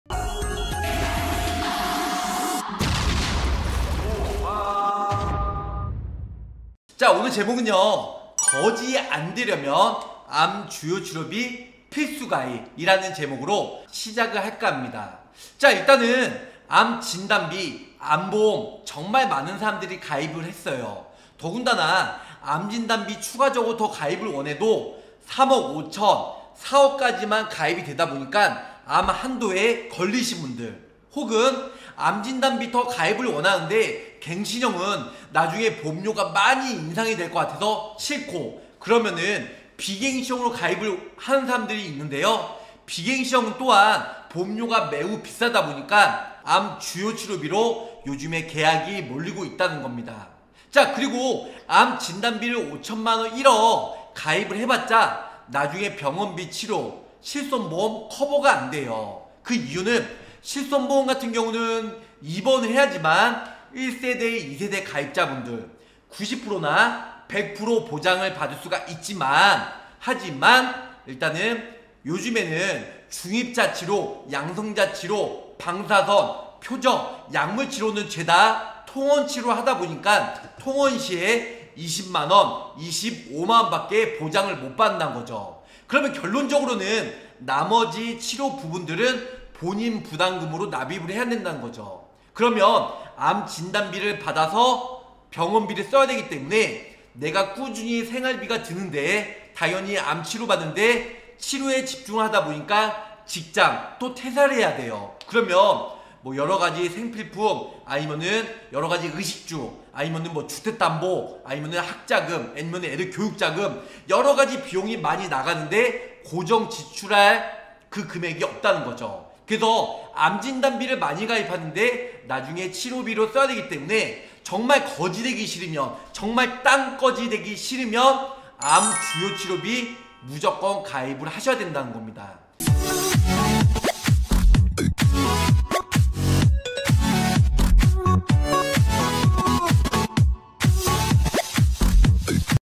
암진단비만 믿으면 집안 풍비박산?? (어마어마한 암치료비용ㅠ)
안녕하세요~~

요즘은 비싼 암진단비보다 저렴하고 가성비좋은
암치료비용으로 많이가입합니다

그중에서도 암주요치료비용이 대세임으로
미리미리 준비하시기 바랍니다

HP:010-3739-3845
카톡친구추가:r2an
카톡플러스친구:보험왕검색